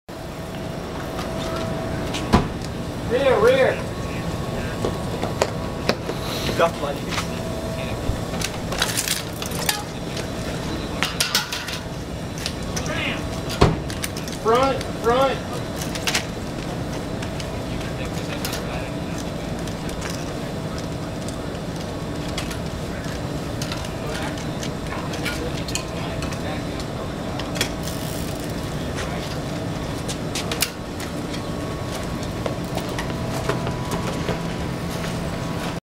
Quick tire change at the Bill Woodul Race Mechanics Clinic
Students at the USA Cycling Bill Woodul Race Mechanics Clinic learn how to change a tire in a road race simulation.